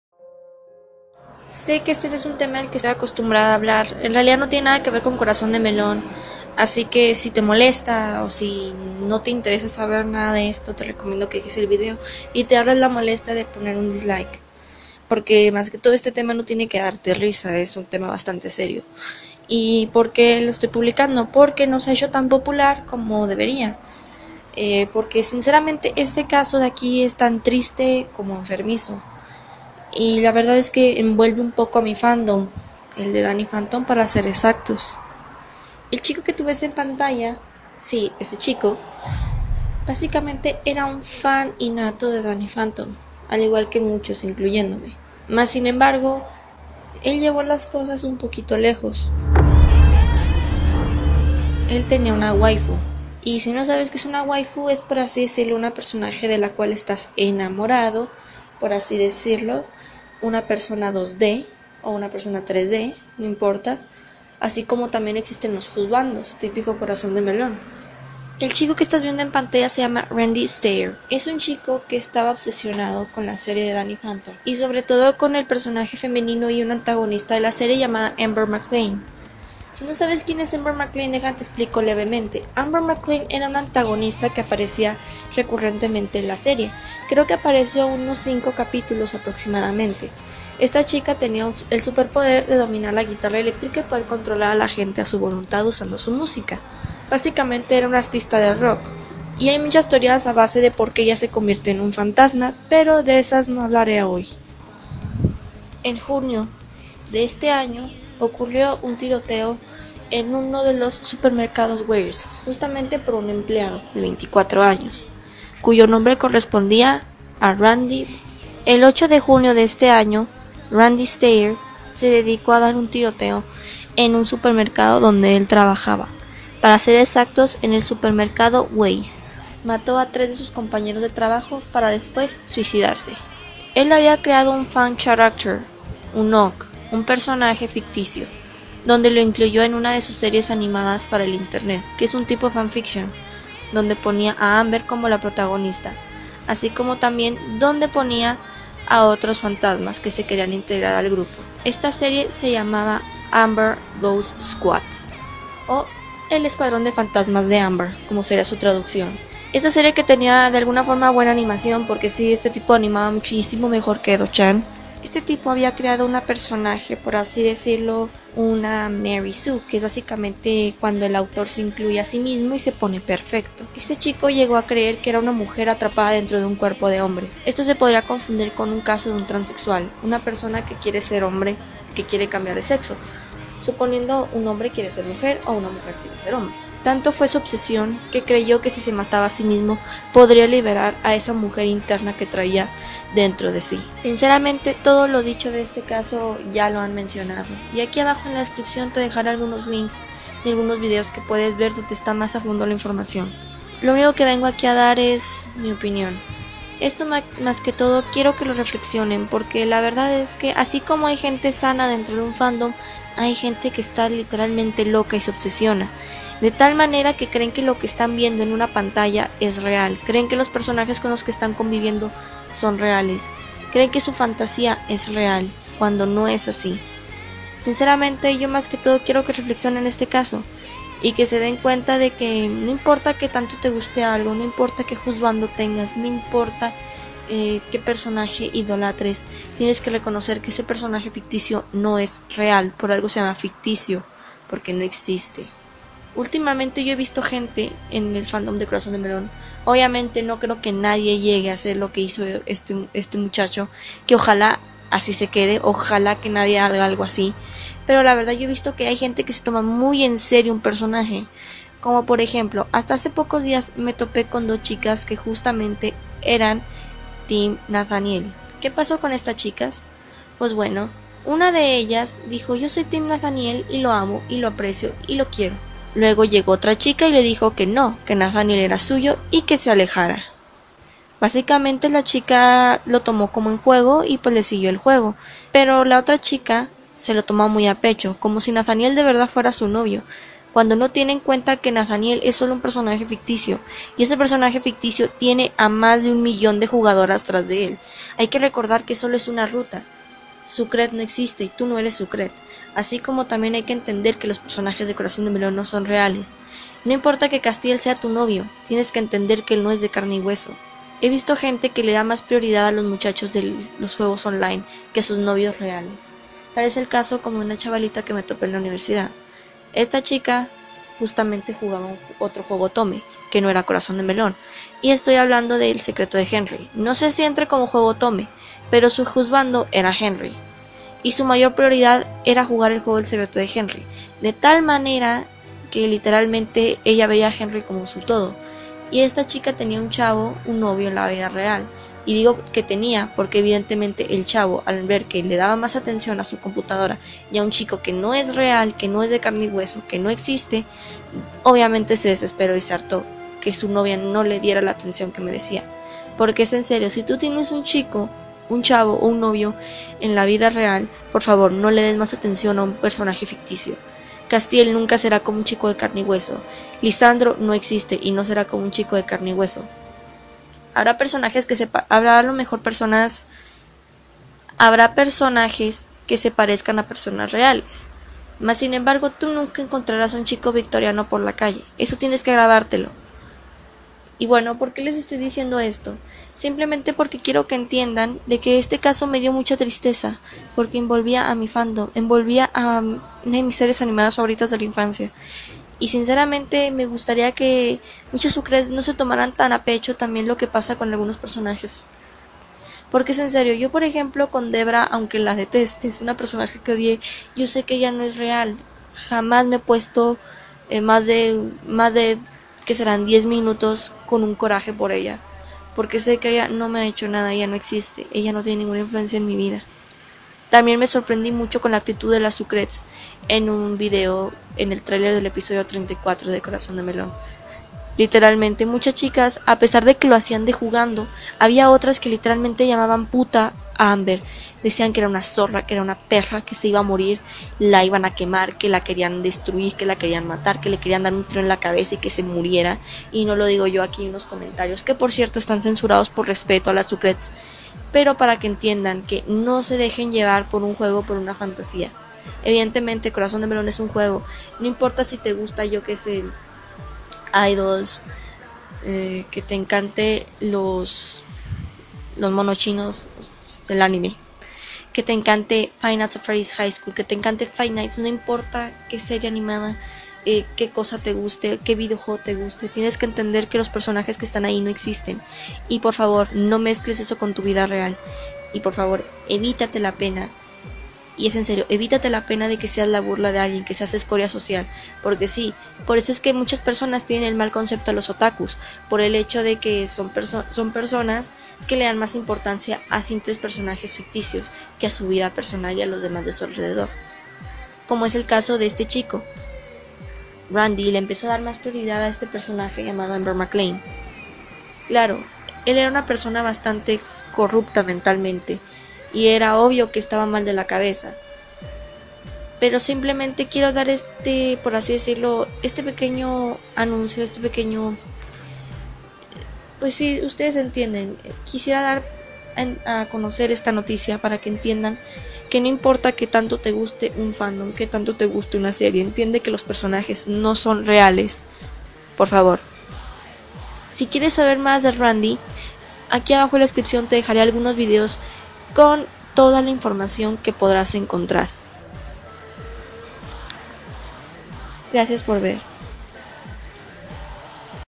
Cuando tu fanatismo puede llegar lejos...(Análisis al caso de Randy Stair)
Nada de lo que esta aquí es de mi propiedad

Espero respeto al memento de comentar y dar sus opiniones prometo volver a mi programación normal de parodias o fanfics después solo quería dar mi opinión al respecto